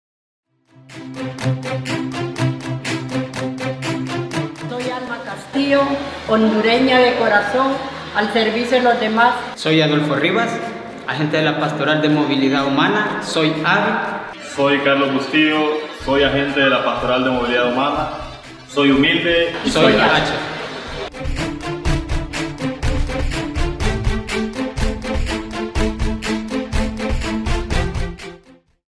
Campaña Soy H. Soy servidor
Dos seres humanos que se miran a los ojos frente a frente, se reconocen y se dan la mano en igualdad de condiciones, construyen una H de Humanidad compartida.
Campaña de la Red Jesuita con Migrantes SoyH ¿y vos?